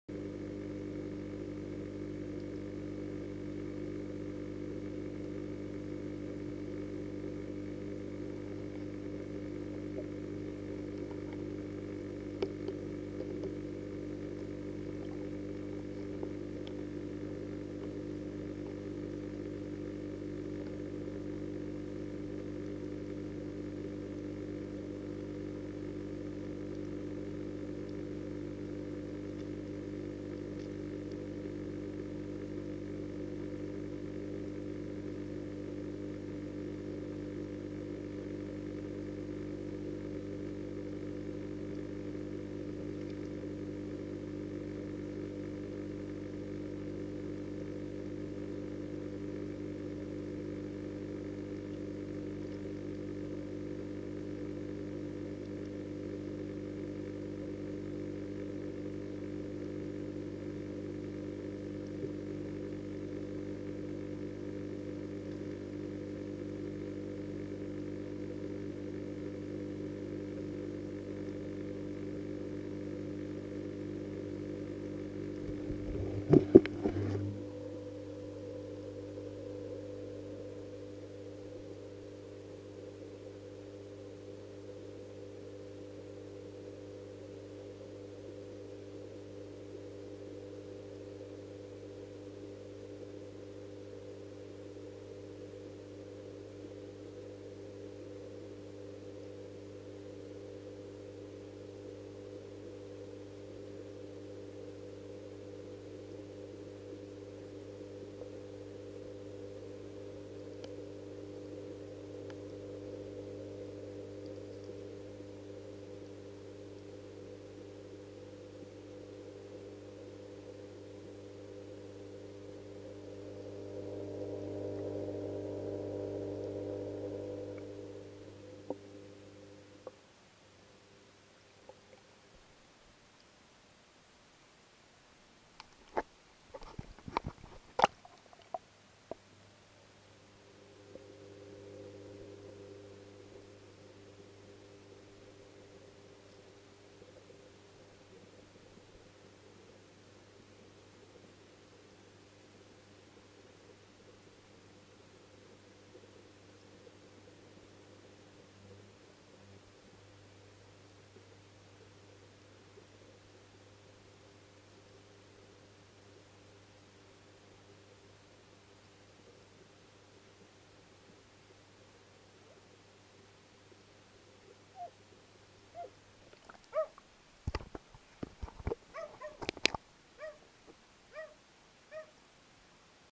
The view the fish have in the pond after installing the bio-filter
I have made a bio-filter for my pond after the fish were starting to swim to much in a pea soup. After making the bio-filter from scratch I set it up as a dry-run next to the pond.
After 2 days the water started to clear. Then we had some rain and the water got a little brown / grey. Two days later it was clear again. I decided to try out my Drift Ghost action cam under the water and get a fish eye view of the pond.
This is the result of that test.